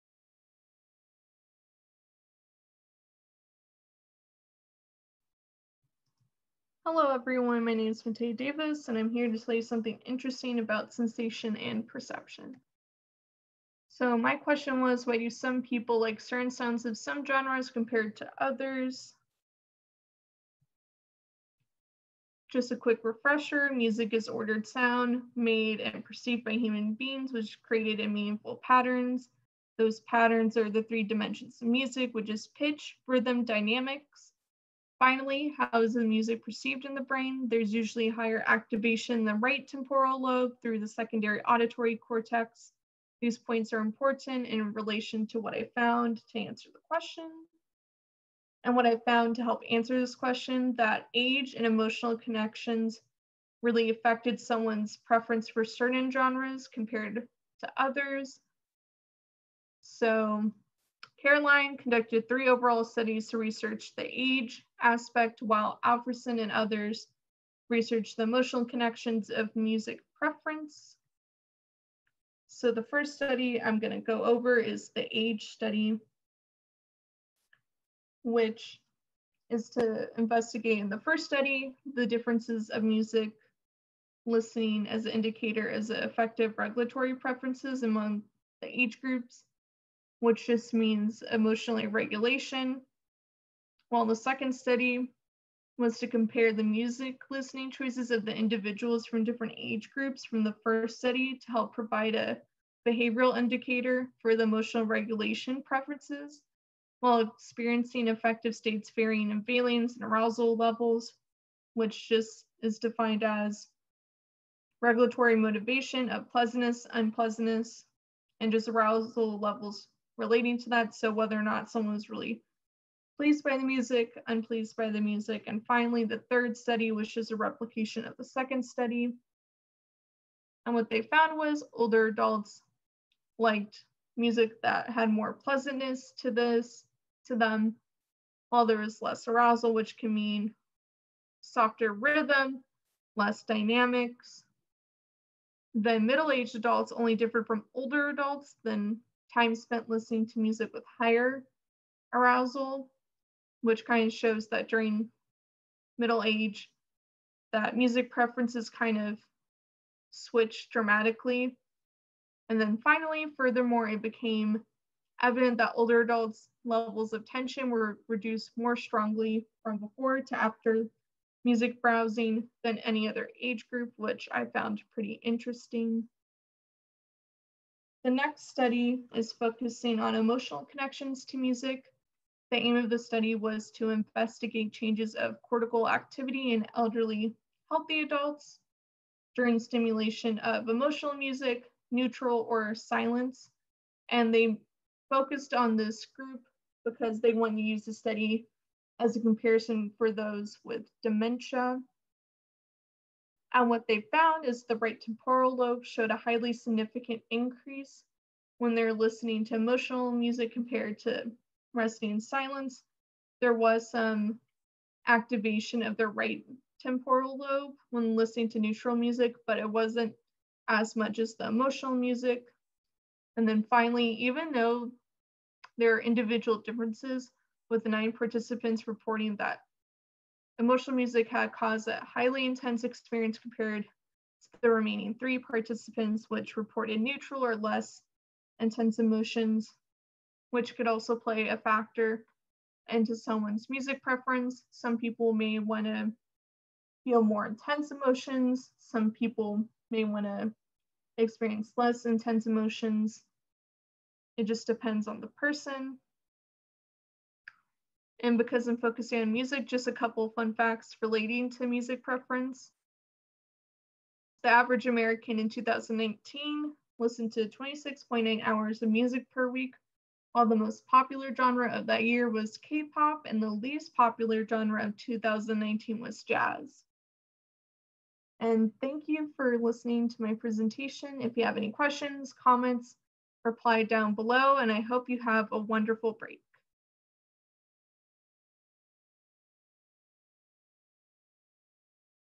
Learnt Tutor Mattea Teaches Sensation and Perception
In this video, Mattea is telling us something interesting about sensation and perception.